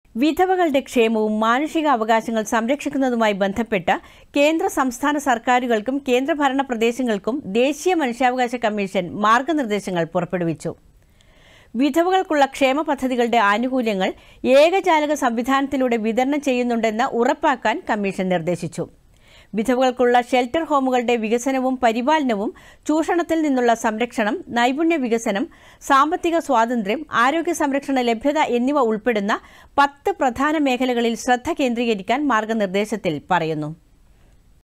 വിധവകളുടെ ക്ഷേമം: ദേശീയ മനുഷ്യാവകാശ കമ്മീഷന്‍ മാര്‍ഗനിര്‍ദേശങ്ങള്‍ പുറപ്പെടുവിച്ചു
വിധവകളുടെ ക്ഷേമവും, മാനുഷിക അവകാശങ്ങള്‍ സംരക്ഷിക്കുന്നതുമായി ബന്ധപ്പെട്ട്‌, കേന്ദ്ര - സംസ്ഥാന സര്‍ക്കാരുകള്‍ക്കും കേന്ദ്ര ഭരണ പ്രദേശങ്ങള്‍ക്കും, ദേശീയ മനുഷ്യാവകാശ കമ്മീഷന്‍ മാര്‍ഗനിര്‍ദേശങ്ങള്‍ പുറപ്പെടുവിച്ചു.

വിധവകള്‍ക്കുള്ള ക്ഷേമപദ്ധതികളുടെ ആനുകൂല്യങ്ങള്‍, ഏകജാലക സംവിധാനത്തിലൂടെ വിതരണം ചെയ്യുന്നുണ്ടെന്ന്‌ ഉറപ്പാക്കാന്‍, കമ്മീഷന്‍ നിര്‍ദ്ദേശിച്ചു.